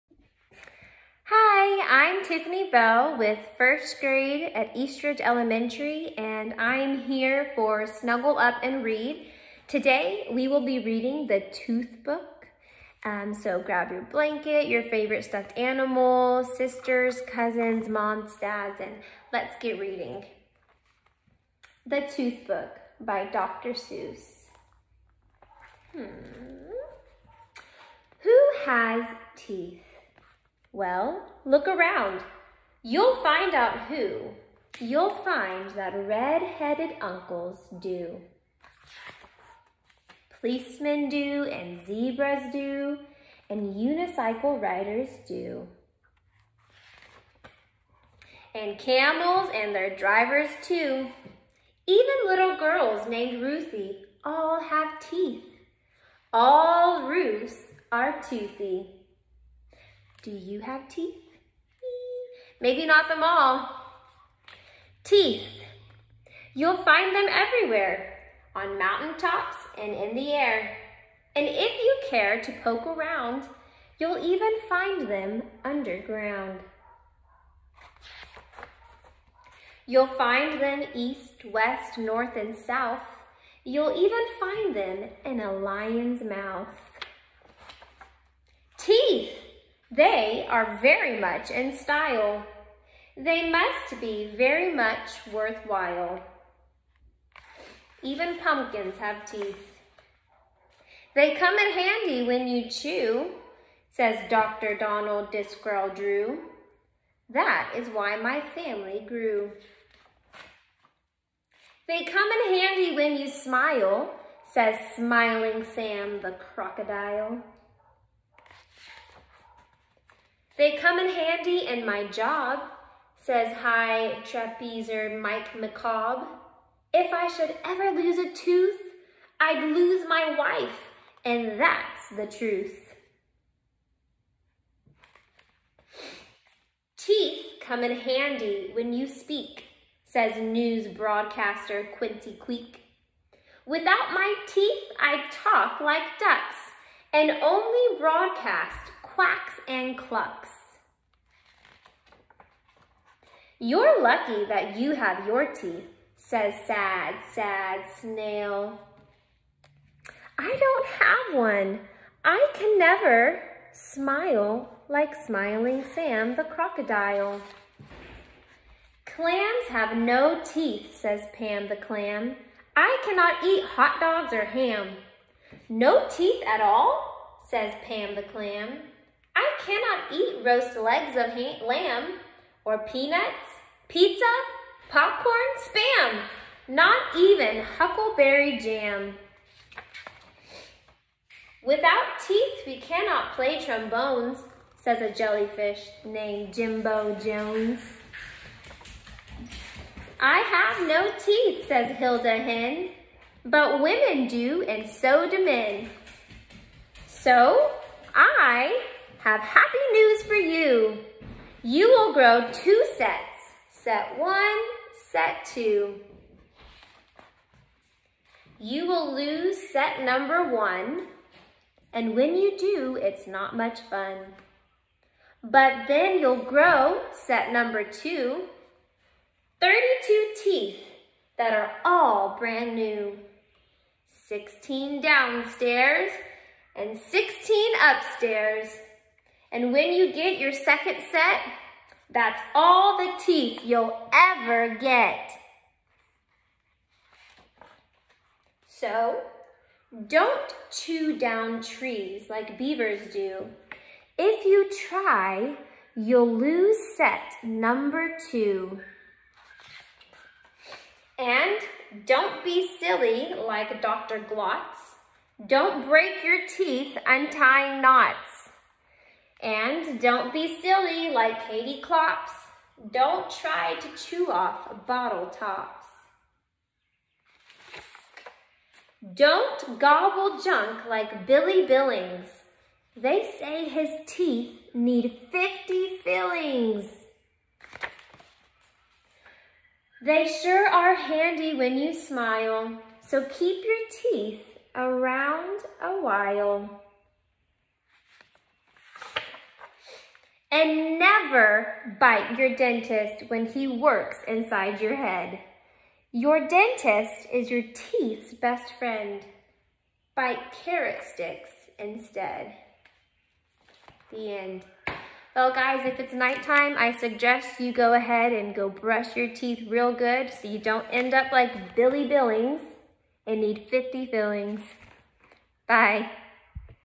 The Tooth Book Read-aloud
LeSieg, Theo. (2017). The Tooth Book. Random House Childrens Books: New York.

Read-aloud by Mrs. Tiffany Bell